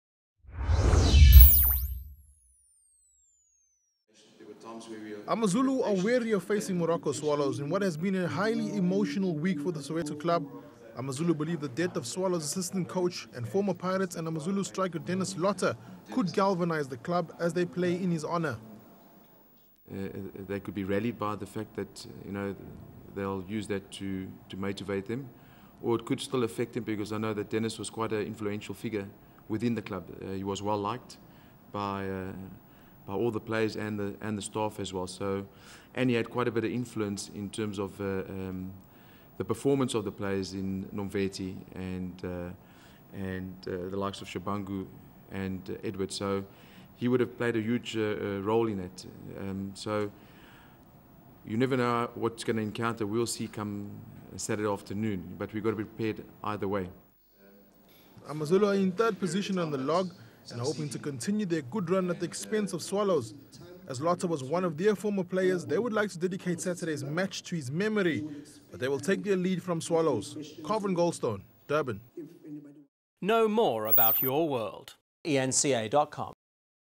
Swallows to honour Dennis Lota in AmaZulu game
Durban - AmaZulu are wary of facing Moroka Swallows in what has been a highly emotional week for the Soweto club.Following the death of Swallows' assistant coach, Dennis Lota, the Birds could be galvanised as they play in his honour.